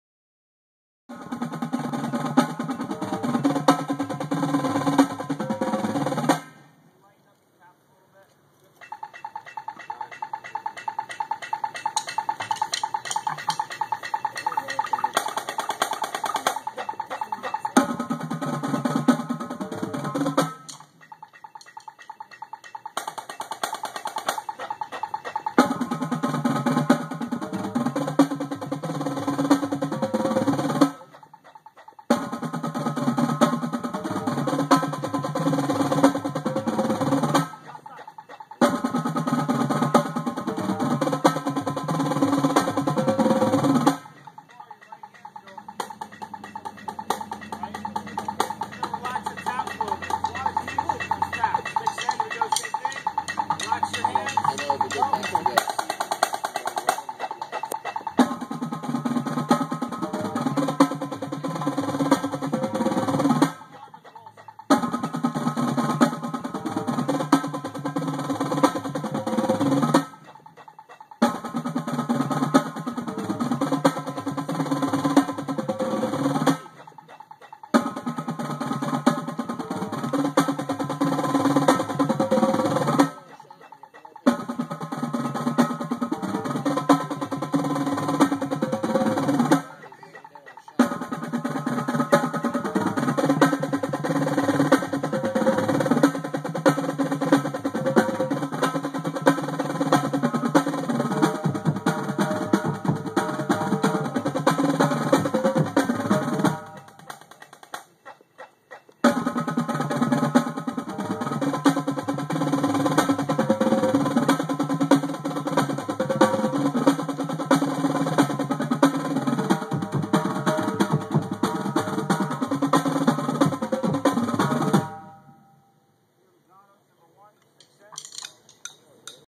2012 Cadets in the lot
This is the cadets, rehearsing their christmas in july themed show.